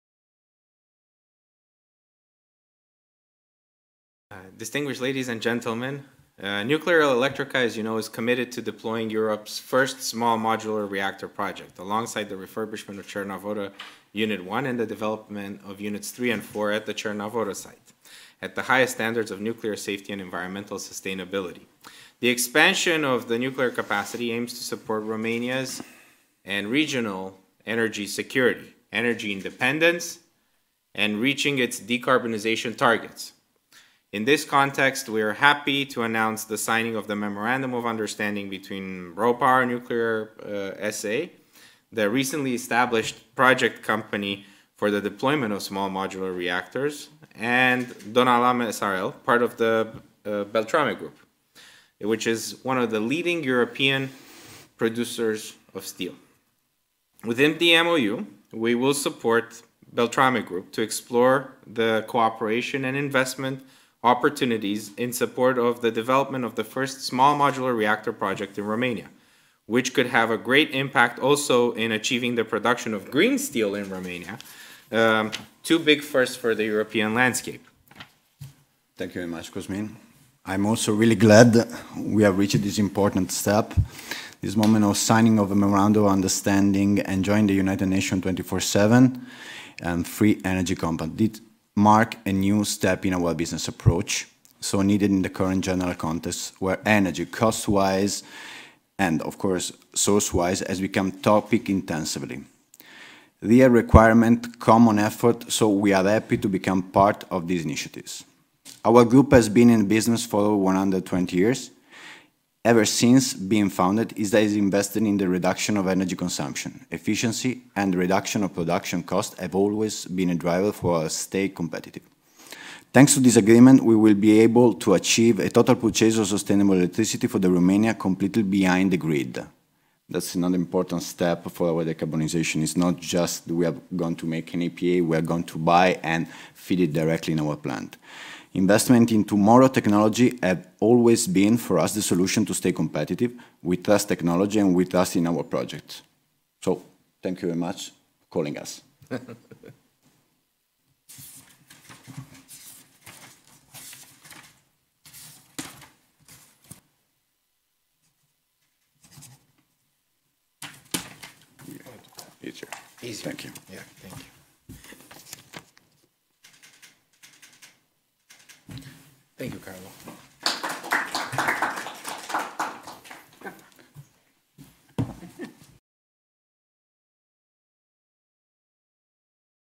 Semnarea Memorandumului de Înțelegere (MoU) între RoPower Nuclear SA si Donalam SRL
SN Nuclearelectrica SA, singurul producător nuclear din România și un contributor strategic la securitatea energetică și la decarbonizarea României, anunță semnarea Memorandumului de Înțelegere (MoU) între RoPower Nuclear SA, compania de proiect recent înființată pentru implementarea reactoarelor modulare mici (SMR), și Donalam SRL, parte a AFV Beltrame Group, lider european în producția de oțel, la pavilionul IAEA Atoms for Climate din cadrul COP 27.